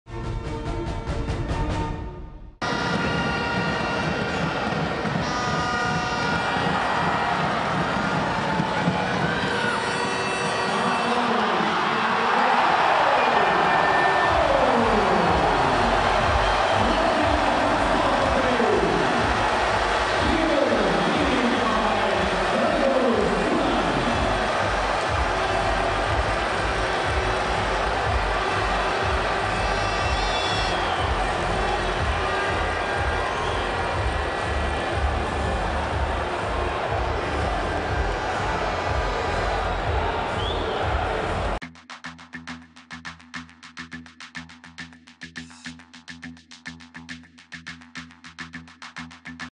Nice steal by Györi's line player Broch | TIPPMIX EHF FINAL4
Just a few seconds before the end of the semi-final's first half Buducnost vs Györi, line player Yvette Broch showed a nice steal at the TIPPMIX EHF FINAL4.